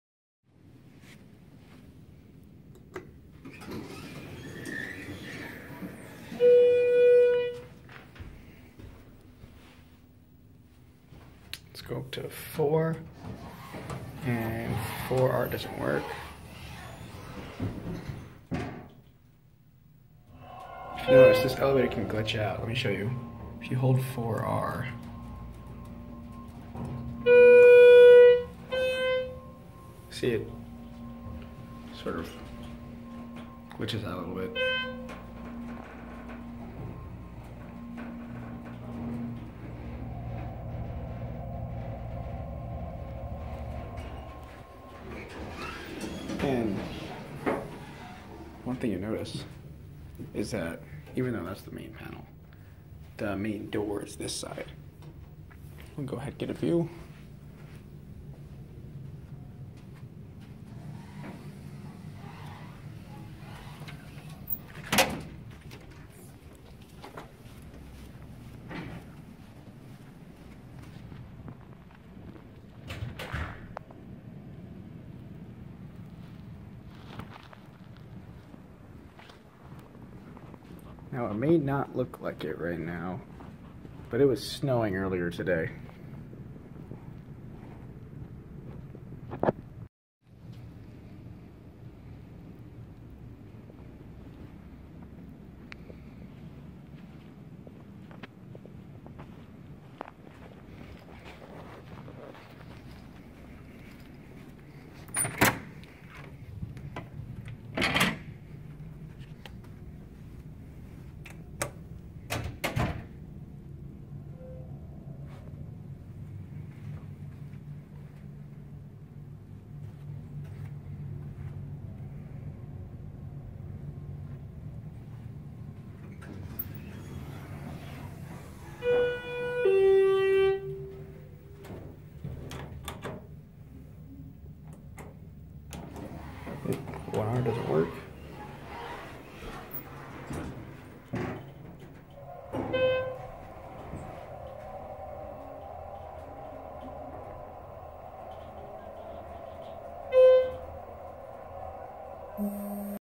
Schindler Hospital-Sized (w/ Call-Cancel Glitch) Elevator at 3601 University Avenue, Grand Forks, ND
Schindler Hydraulic Hospital Sized Elevator at 3601 University Avenue, Grand Forks, ND